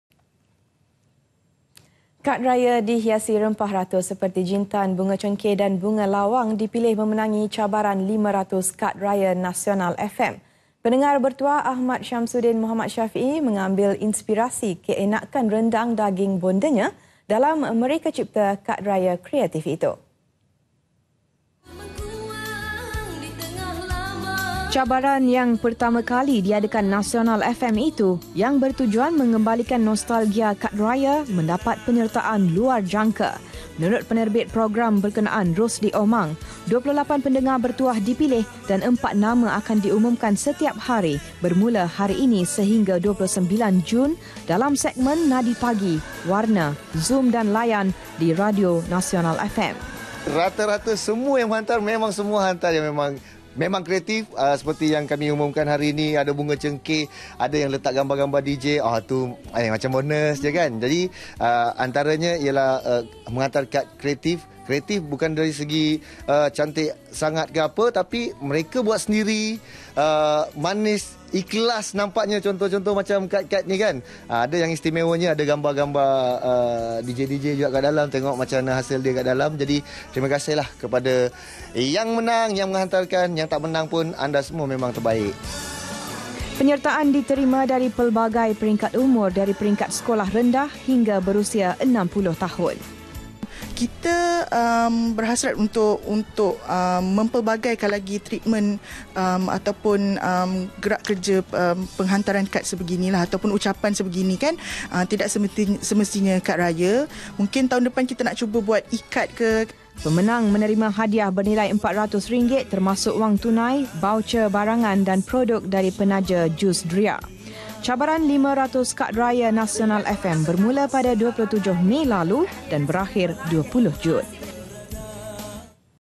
CABARAN 500 KAD RAYA NASIONALFM - KAD RAYA AHMAD SHAMSUDDIN PALING KREATIF [23 JUN 2017]
Kad raya dihiasi rempah ratus seperti jintan, bunga cengkih, dan bunga  lawang; dipilih memenangi Cabaran 500 Kad Raya Nasional FM .

Pendengar bertuah... Ahmad Shamsuddin Mohd Shafie mengambil inspirasi keenakan rendang daging bondanya,  dalam 'mereka-cipta' kad raya kreatif itu.